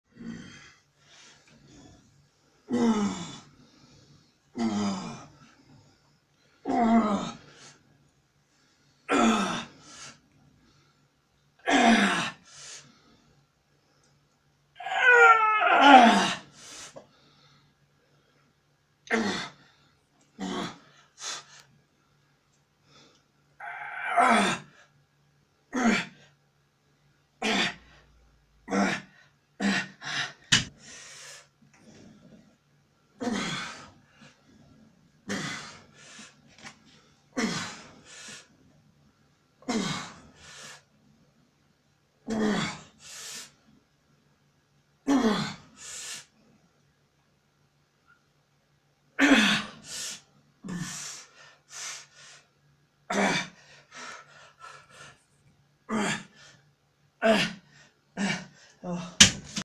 Single arm crossover cable biceps curls 24.3.17
Last week of these so hit an extra 10lb on the stack